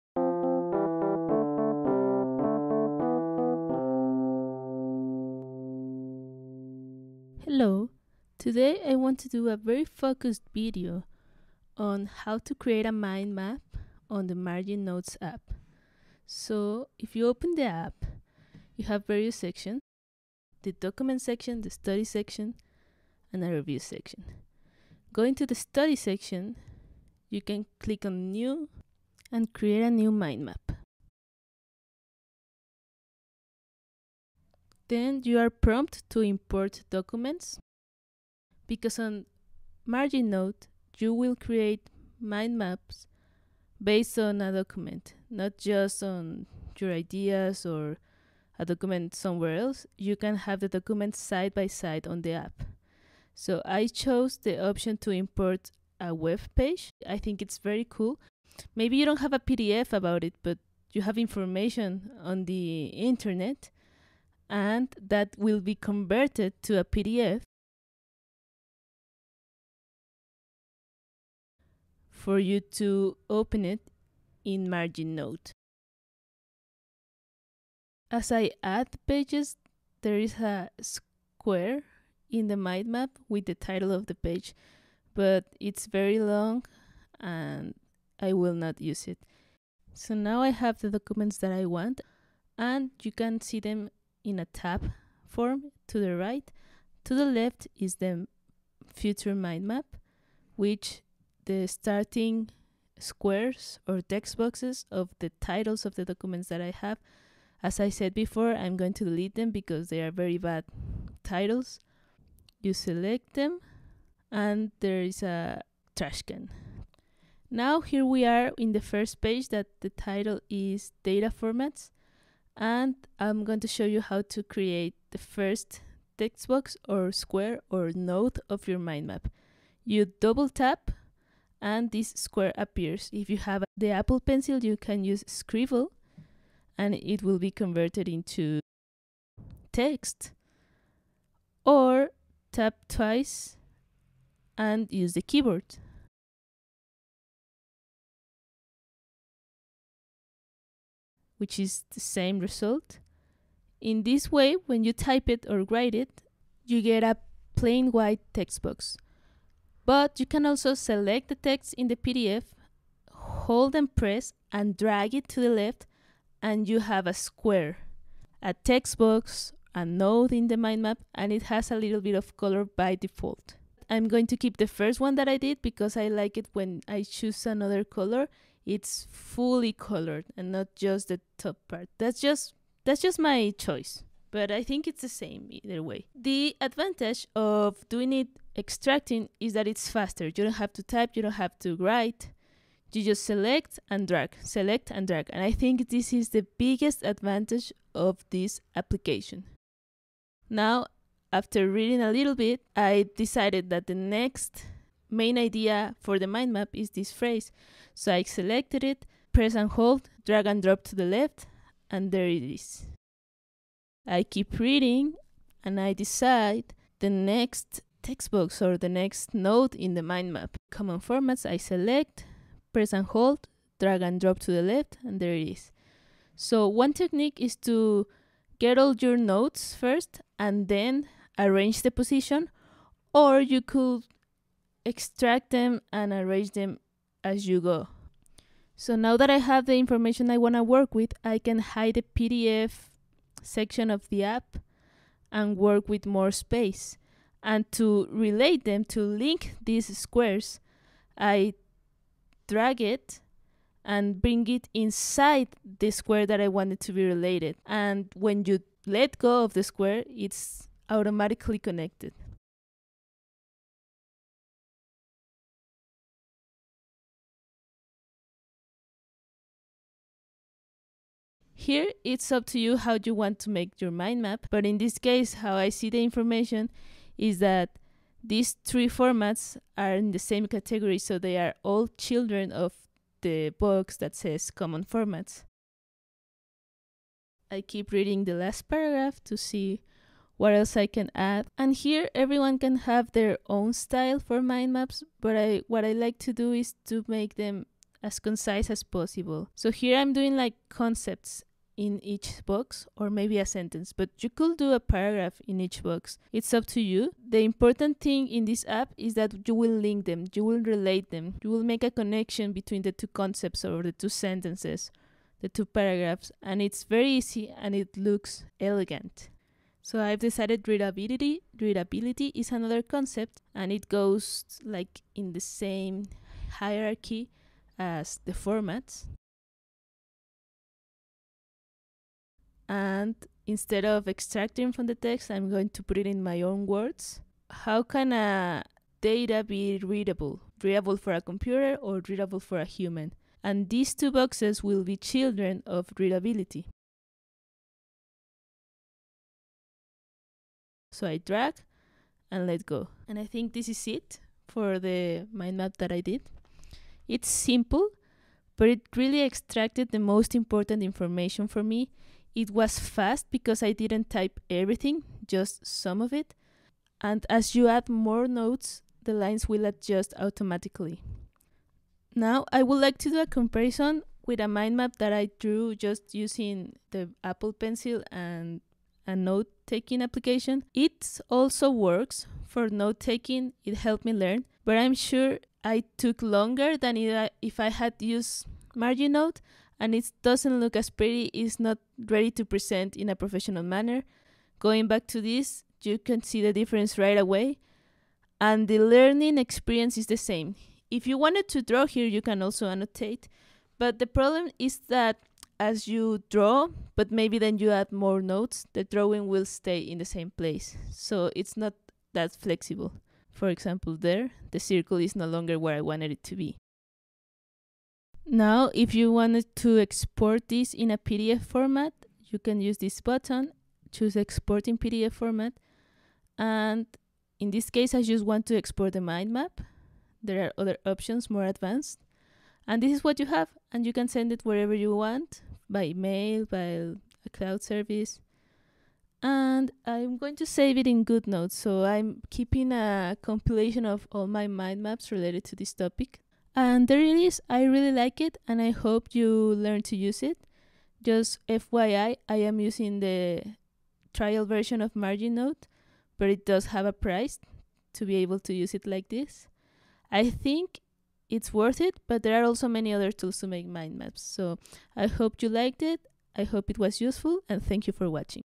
How to make a mind map on Margin Note (for Ipad)
Margin Note tutorial for beginners.

But she is very advanced using it, I tried to make a beginner tutorial.
I hope you all find it useful.